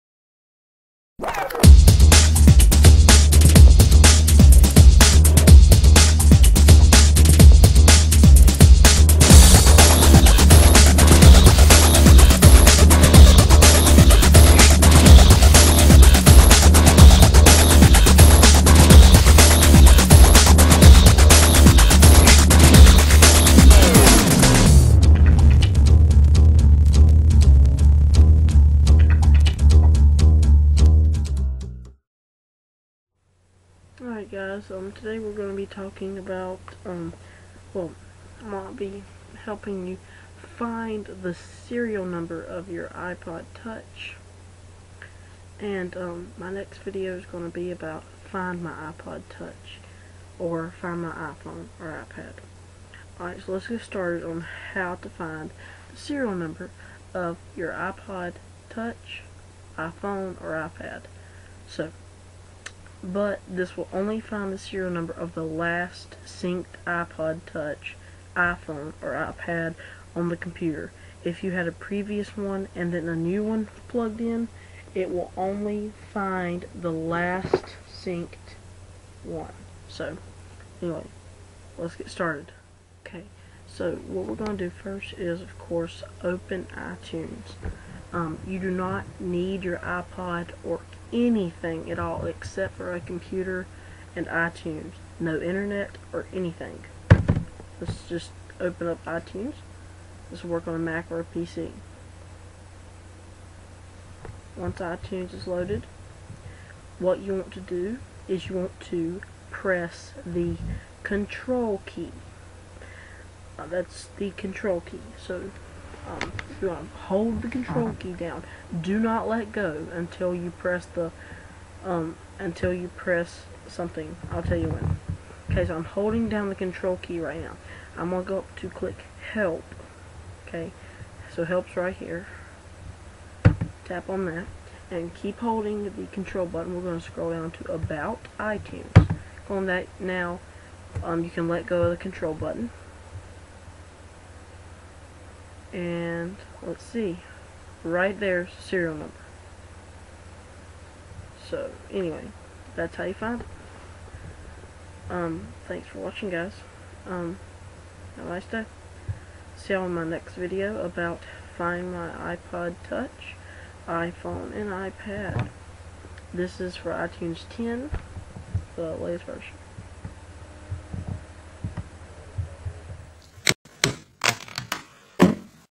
How To Find lost iPhones Serial Number
Oh No! You have lost your iPod, iPhone, or iPad and think or know some-one stole it? Well find your simple serial number in iTunes without anything except a computer with iTunes!    Meanning- No iPod, No Cords, No Wifi, No Internet, No Cards, AND ITS FREE! Its Just in iTunes (iTunes 10 is the version im using.) yt:stretch=4:3 yt:crop=16:9 yt:quality=high